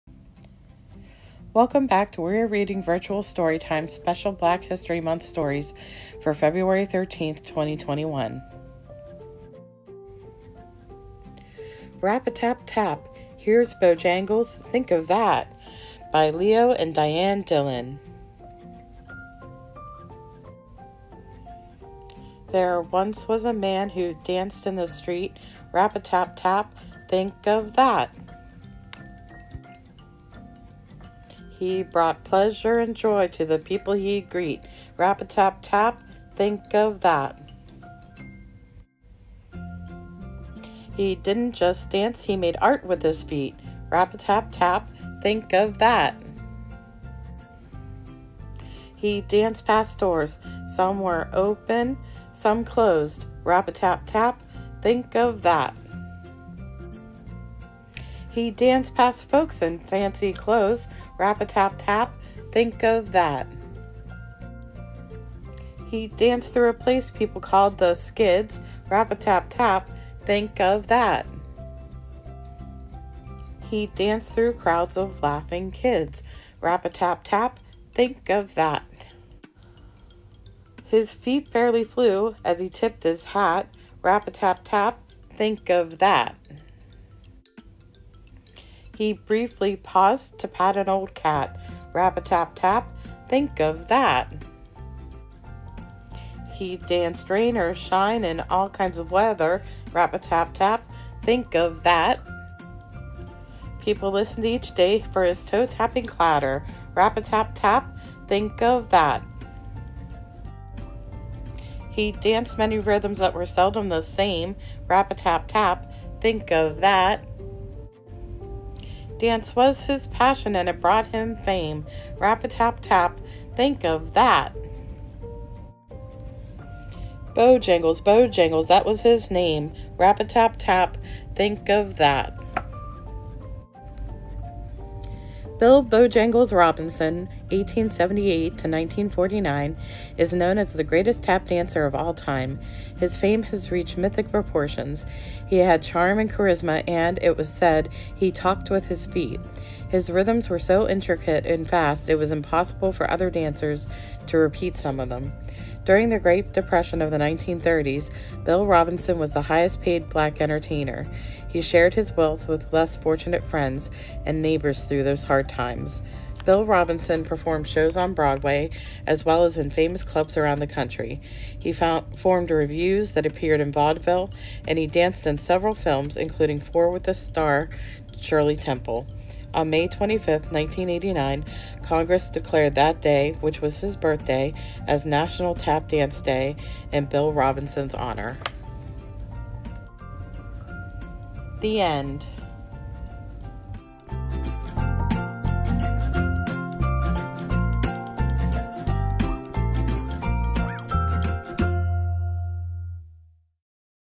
Rap A Tap Tap (Leo & Diane Dillon)
WEIR Reading Virtual Story Time
Black History Month Stories 2/13/21
Rap A Tap Tap: Here’s Bojangles—Think of That! (Leo & Diane Dillon)